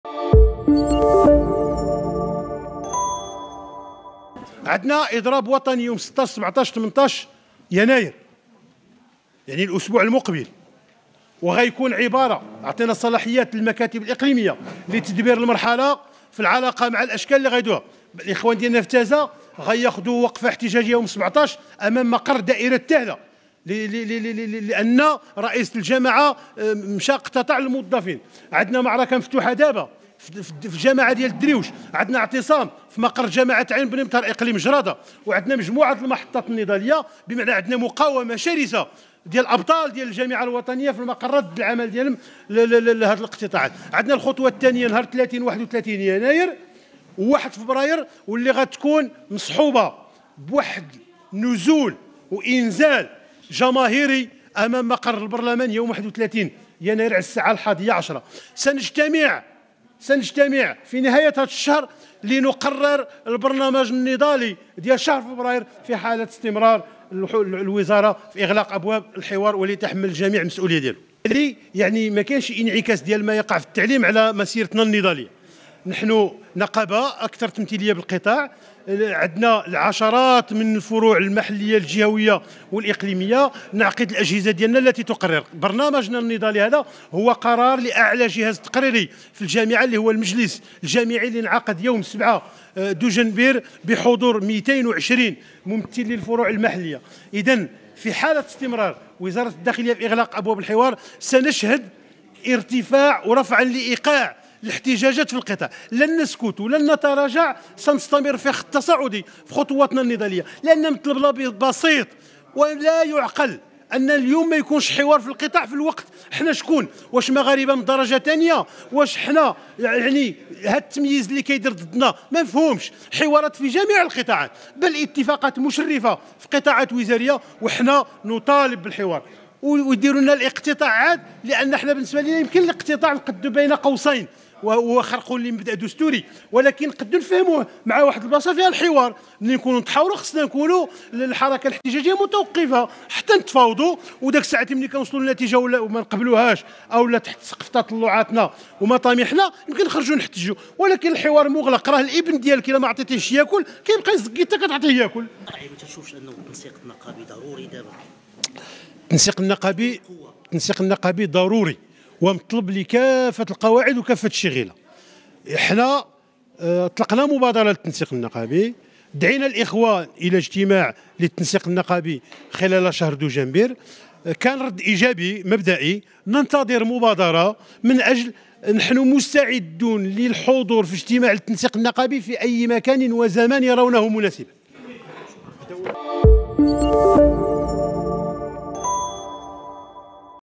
القلعي: نحتج على وزارة الداخلية التي أغلقت باب الحوار في وجه موظفي الجماعات
قررت الجامعة الوطنية لموظفي الجماعات الترابية والتدبير المفوض"UMT" خوض إضراب وطني 16-17-18يناير 2024،مع الحضور لمقرات العمل والانسحاب وخوض أشكال احتجاجية حسب وضعية كل اقليم. وإضراب وطني أيام 30-31 يناير و1 فبراير 2024،مع وقفة احتجاجية أمام البرلمان يو 31 يناير الجاري.
الإعلان على قرار الإضراب جاء خلال الندوة الصحافية التي احتضنها المقر المركزي للاتحاد المغربي للشغل بالدار البيضاء يوم الجمعة 12 يناير 2024، حيث كشفت فيه الجامعة عن وضع اللمسات الأخيرة لرفع دعوة قضائية حول عدم شرعية الاقتطاعات من الرواتب بسبب الإضراب.
واعتبرت الجامعة الوطنية لموظفي الجماعات الترابية، أن الاقتطاعات التي أقرتها وزارة الداخلية هو تضييق على العمل النقابي، كما يعد مخالفة قانونية سقطت فيها الوزارة حيث يسجل تجاوز الداخلية لمبدأ التدبير الحر وفرض تنفيذ إجراء غير قانوني على رؤساء الجماعات.